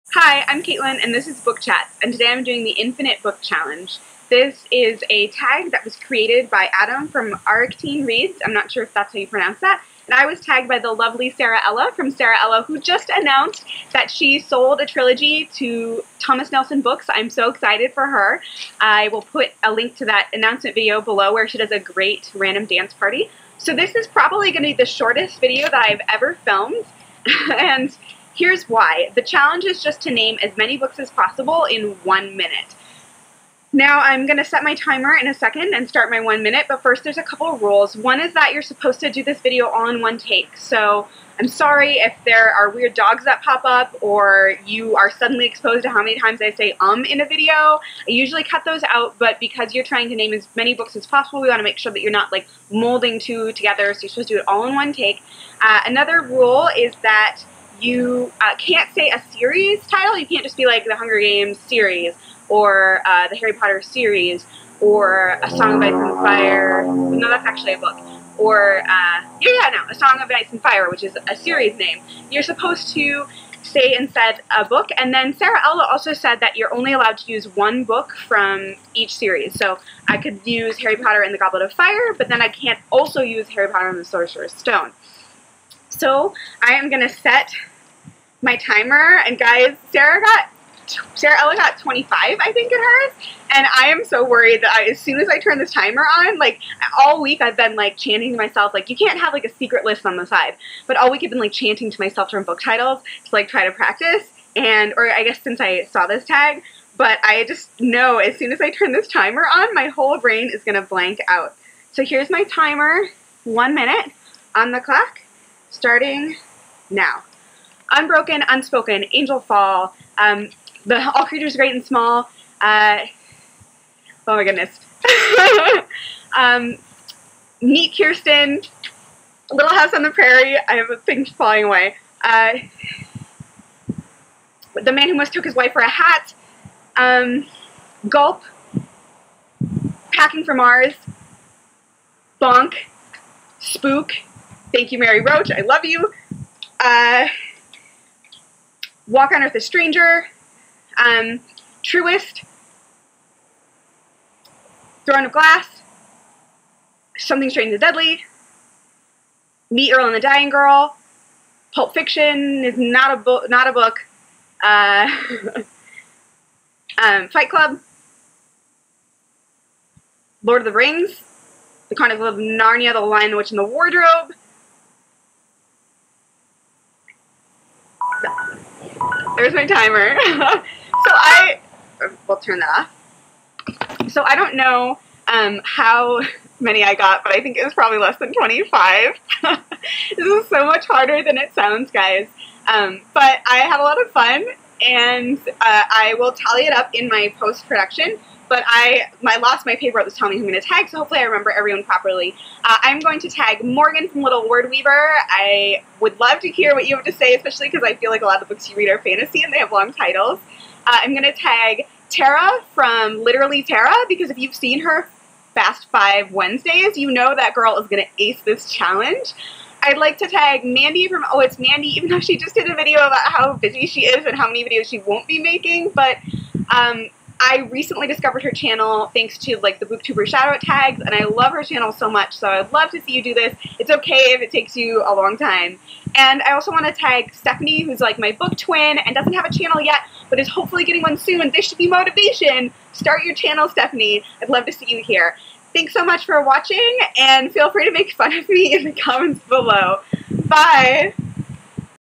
Infinite Book Challenge
Quick note: I haven't read all of the books I mention in this video.

I was tagged by the lovely Sara Ella who just announced her book deal!

Also! I'll be back this Wednesday with a Top 5 Wednesday video and I'm hoping to launch a new series of videos on Friday.  Thanks for sticking with me even with my irregular posting schedule.